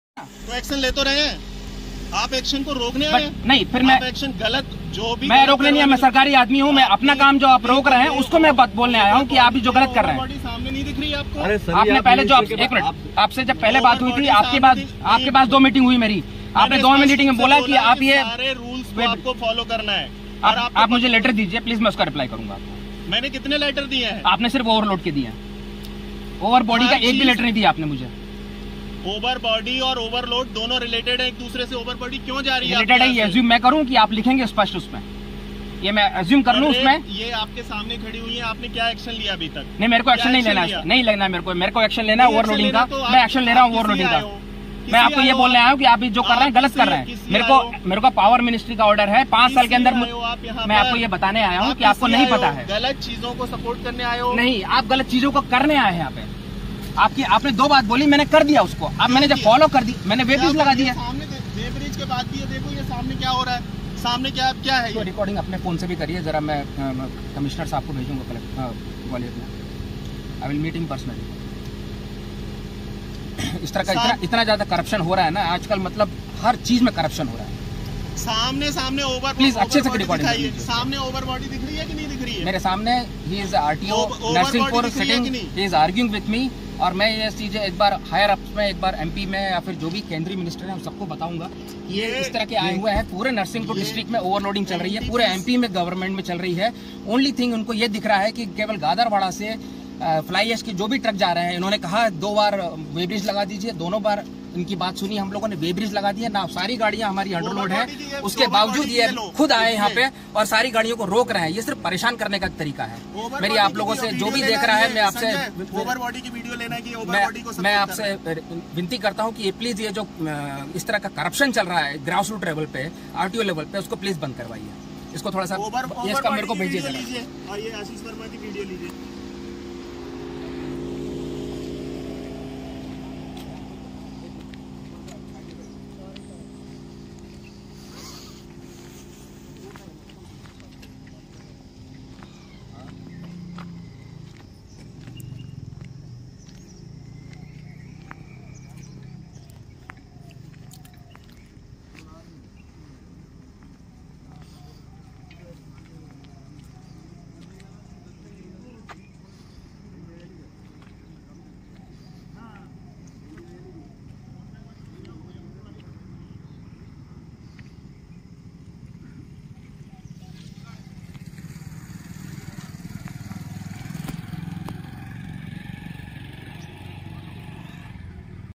RTO अधिकारी व NTPC अधिकारी की जुबानी एक दूसरे को गलत बताने की कहानी को देखें और भ्रष्टाचार को समझे
narsinghpur RTO अधिकारी व NTPC अधिकारी की जुबानी एक दूसरे को गलत बताने की पूरी कहानी जरूर देखें